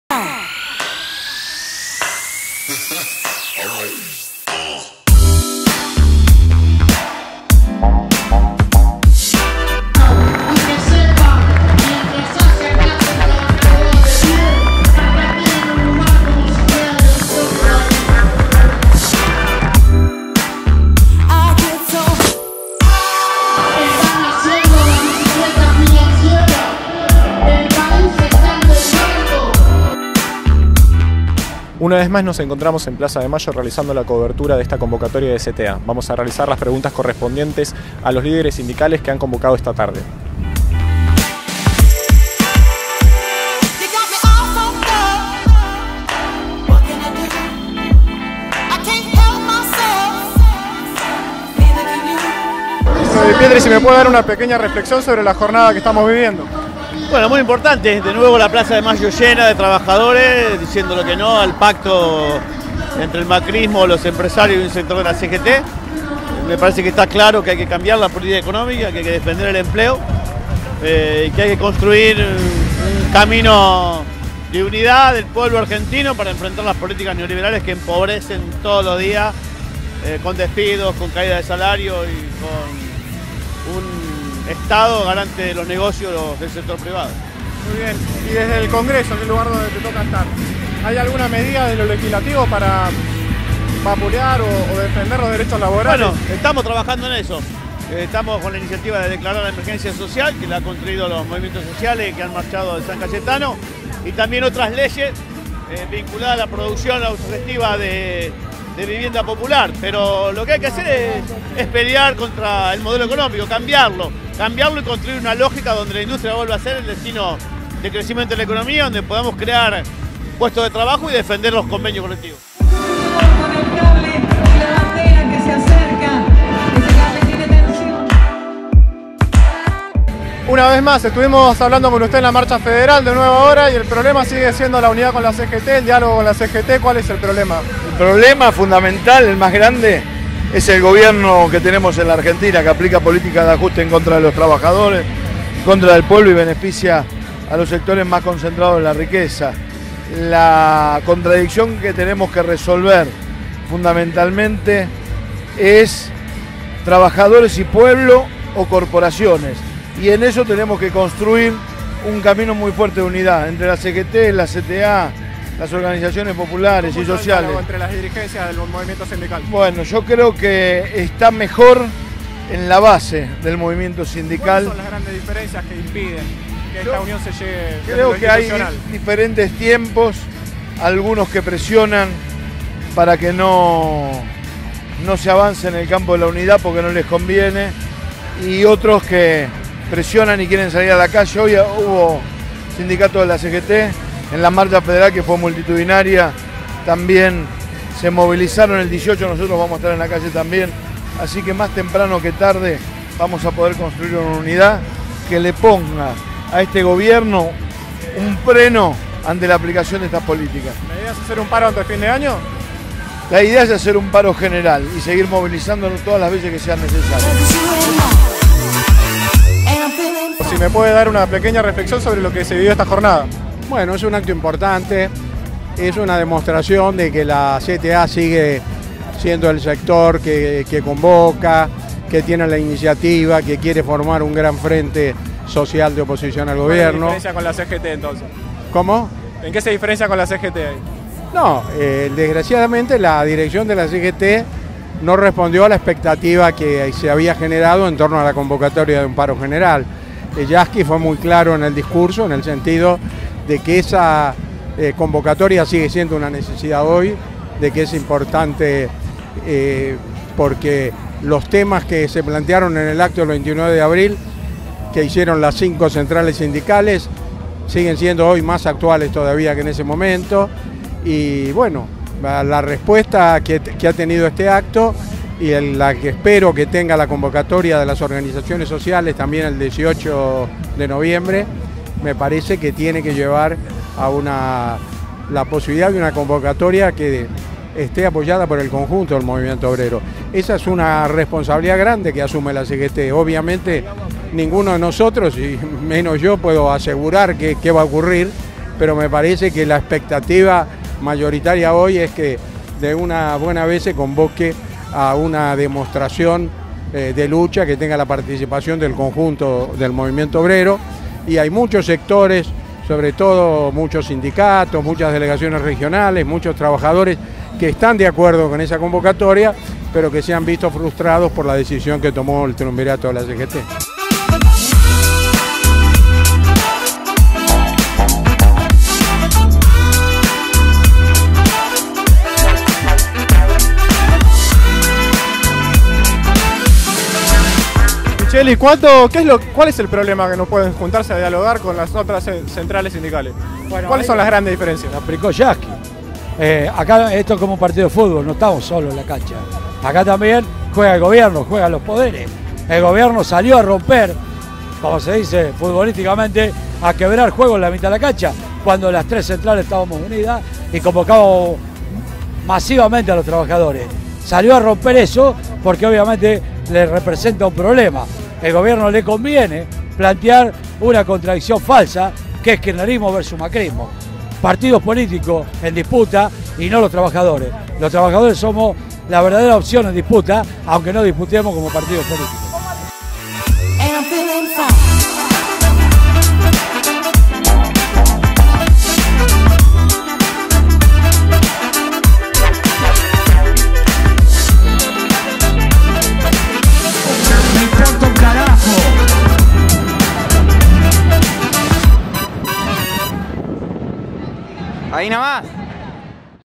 Marcha CTA 4-11-16
Cobertura de la Corresponsalía de NKRA Producciones en Buenos Aires, sobre la Marcha convocada por la CTA el 4-11-16